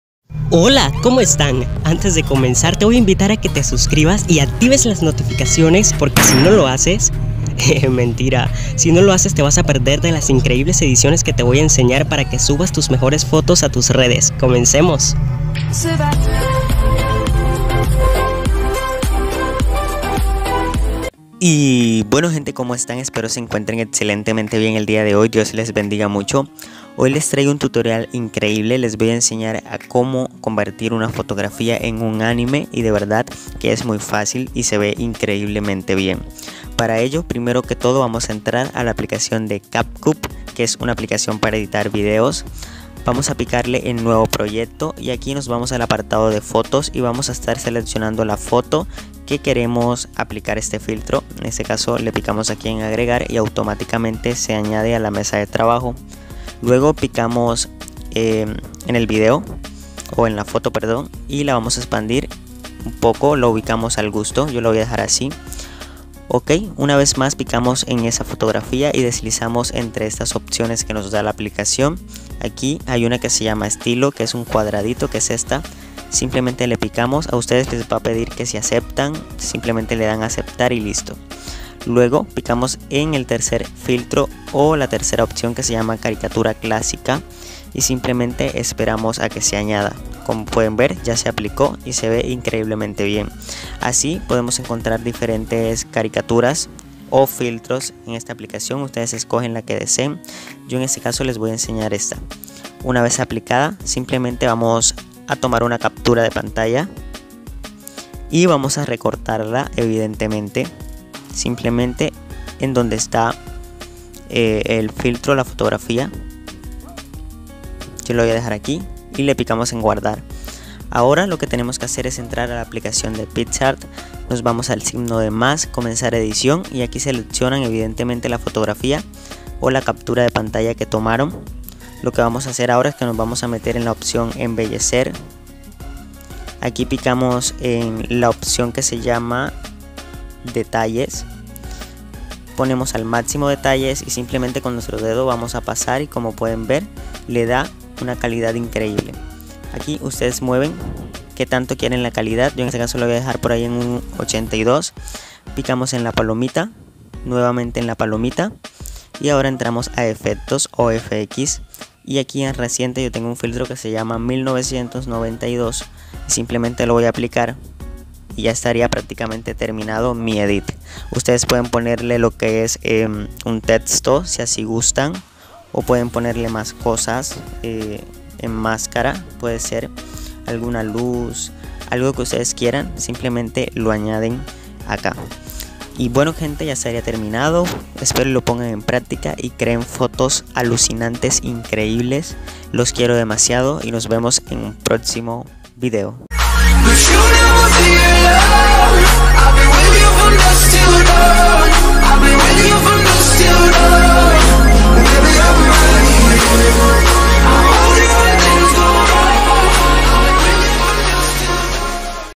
❣️como convertir tu FOTO en ANIME con PICSART y CAPCUT// PICSART TUTORIAL *FACIL
❤️LOS AMOOO!!❤️

❤️Dirección correo postal :
calle 10 A #30-71 Sincelejo -sucre Colombia

como editar en picsart aesthetic